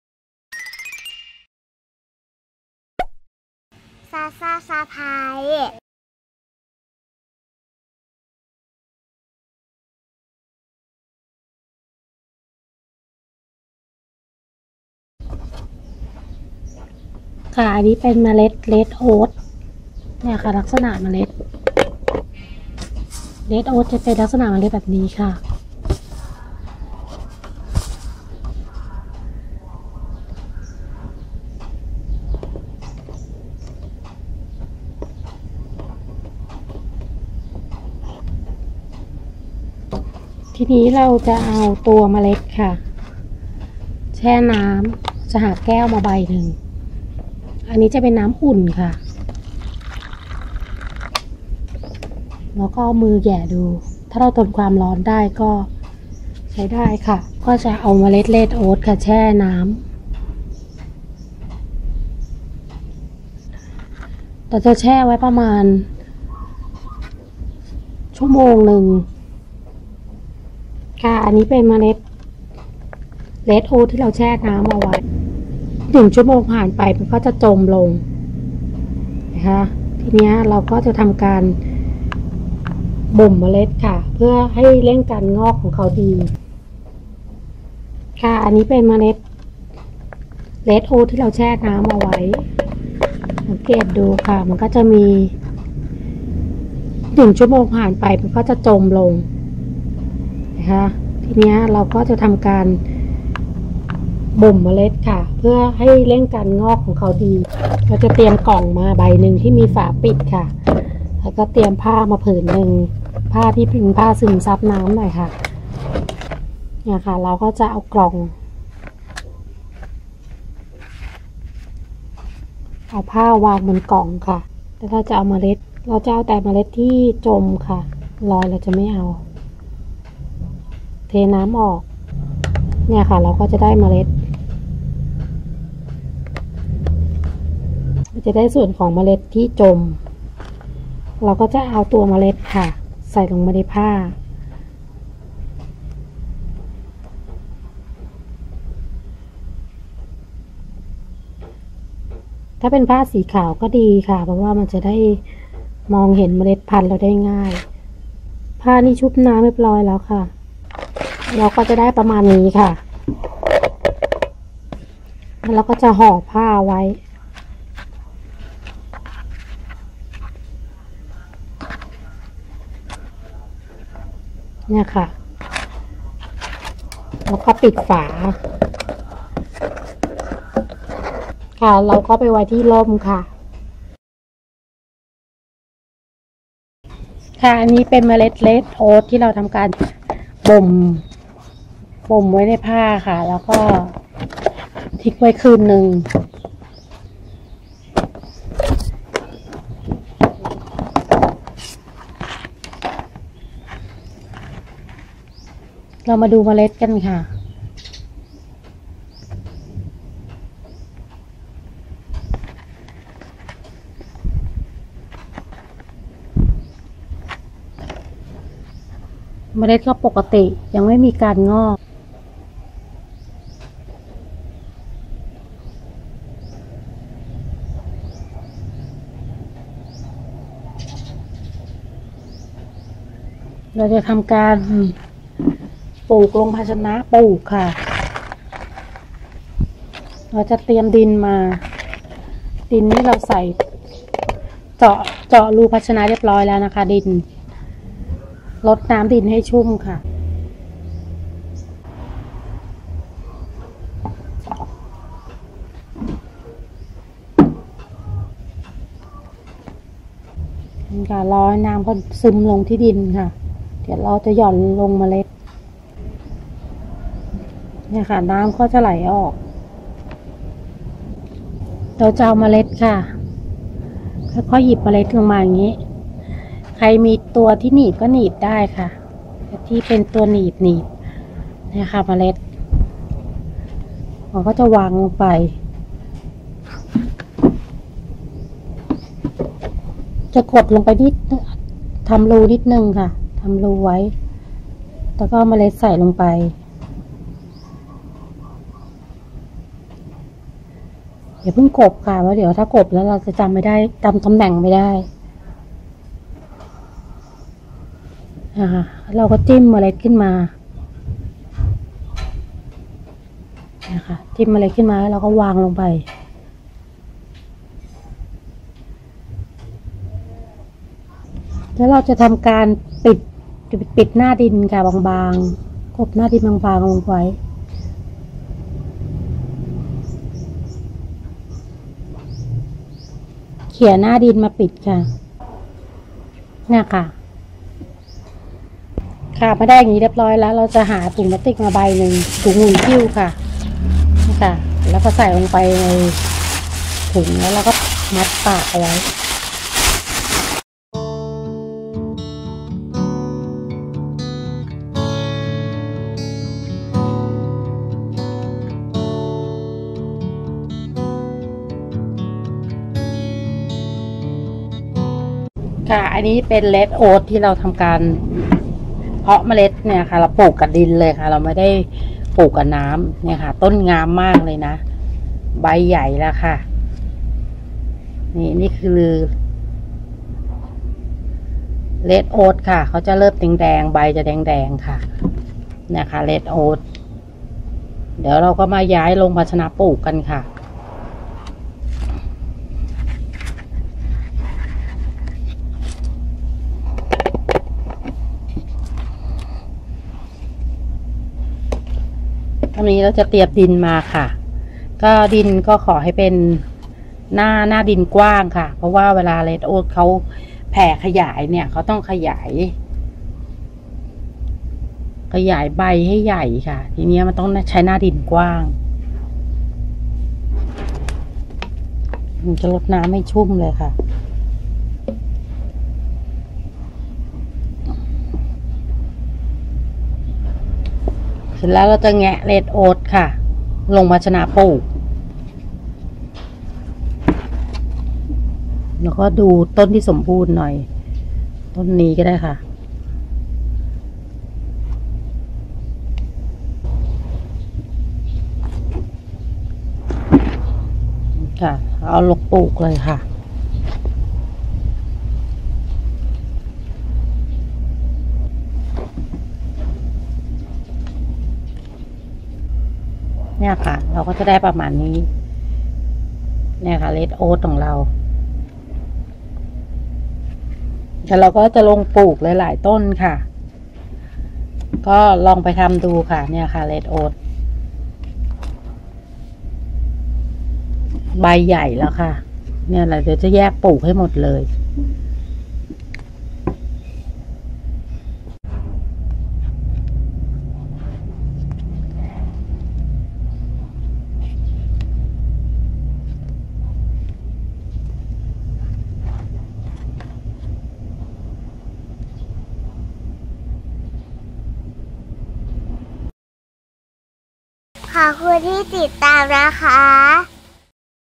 red oak lettuce salad | วิธีปลูกผักสลัดเรดโอ๊ด ปลูกดิน ต้นไม่ยืด อ้วนแข็งแรง
วิธีเพาะเมล็ดผักสลัดเรดโอ๊ดลงดิน ต้นไม่ยืด  แข็งแรง

อยากดูการเพาะปลูก เพาะเมล็ด แจ้งไว้ในคอมเม้นไว้ค่ะ

📌 Google+ : SasahzSurprise